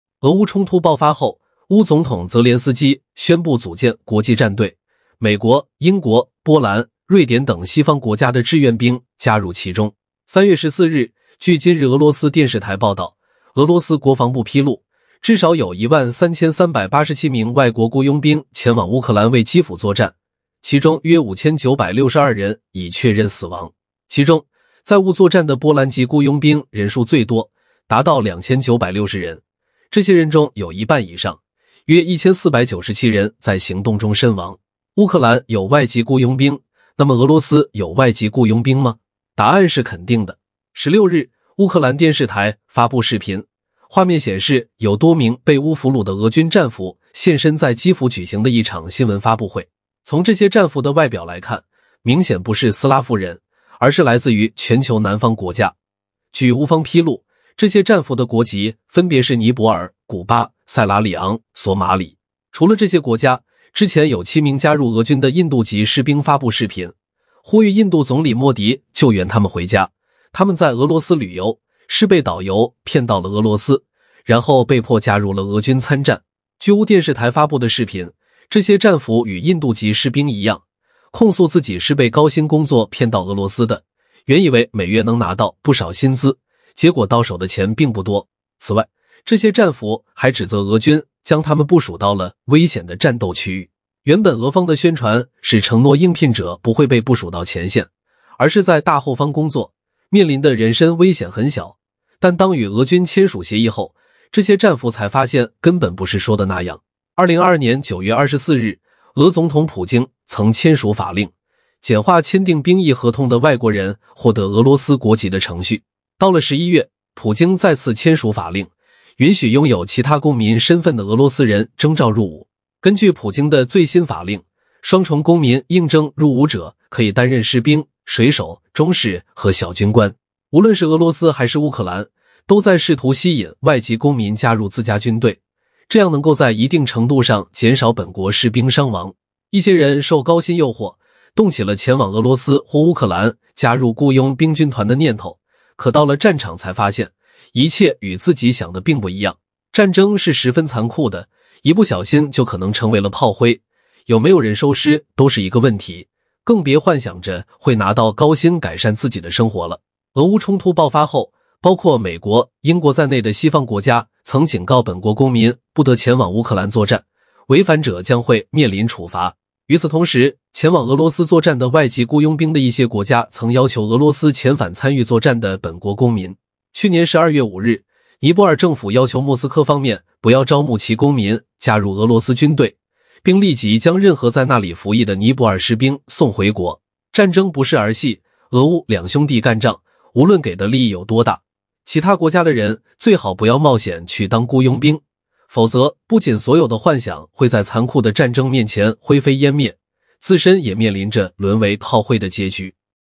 乌克兰雇佣兵来自西方，俄外籍士兵来自哪里呢？乌电视台公布画面
俄乌冲突爆发后，乌总统泽连斯基宣布组建“国际战队”，美国、英国、波兰、瑞典等西方国家的志愿兵加入其中。3月14日，据今日俄罗斯电视台报道，俄罗斯国防部披露，至少有13387名外国雇佣兵前往乌克兰为基辅作战，其中约5962人已确认死亡。